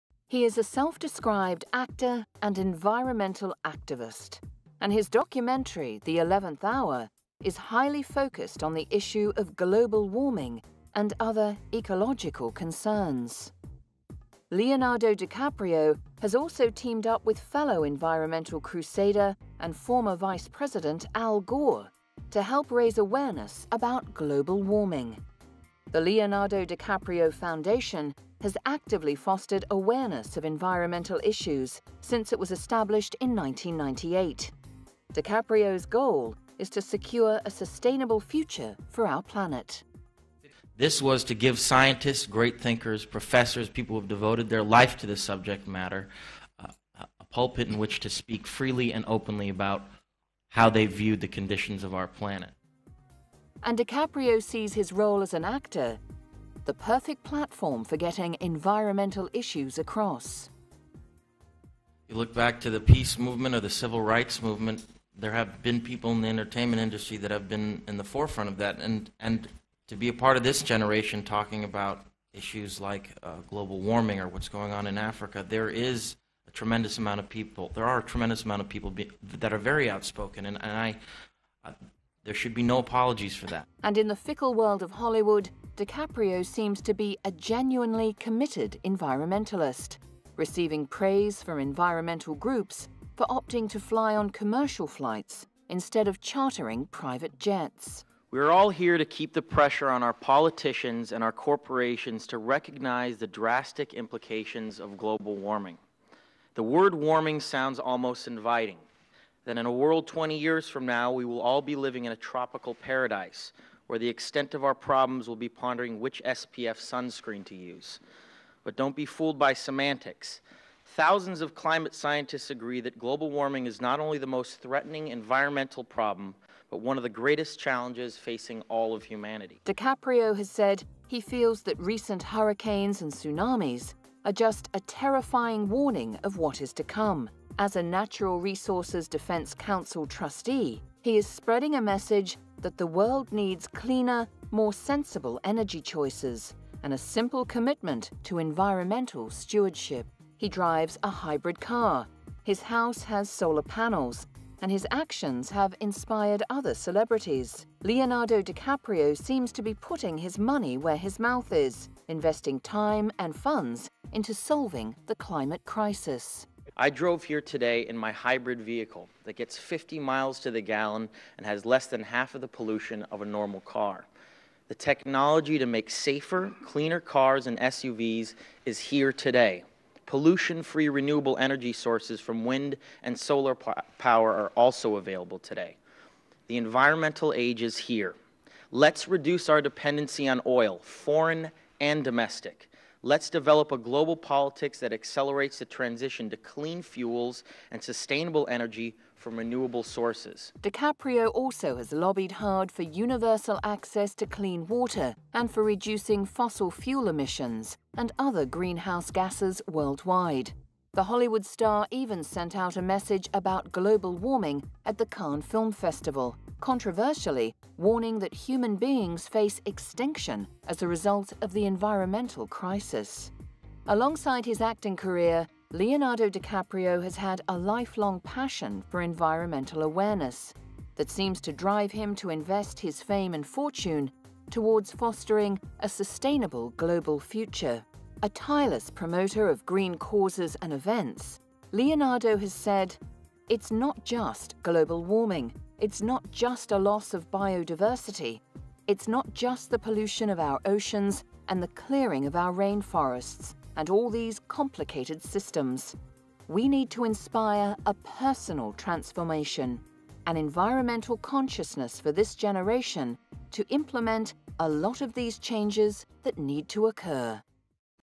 Leonardo DiCaprio - Oscar Winning American Actor | Biography Of Famous People
Witness the Popular Story of the World Famous Actor, and Environmentalist "Leonardo DiCaprio" (Famous Personalities, Biography Of Famous People, Story For Kids, Documentary, Biography).

About Leonardo DiCaprio

Leonardo DiCaprio – He describes himself as an actor and an activist. His documentary, The Eleventh Hour, strongly focuses on global warming and he set up the Leonardo DiCaprio Foundation to promote awareness of ecological issues such as climate change. Part of his platform is to convince celebrities to forgo their private jets in favour of commercial flights, to reduce their carbon footprint.